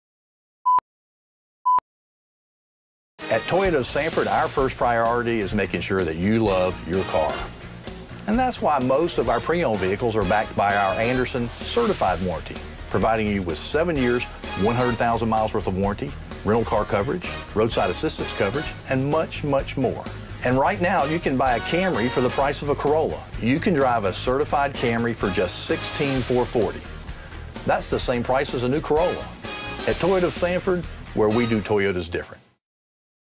Anderson Certifed Warranty
Come in and buy Camrys for the price of Corollas! Almost all of our pre-owned vehicles have this warranty and you certainly shouldn't pass up the chance to get your hands on one today!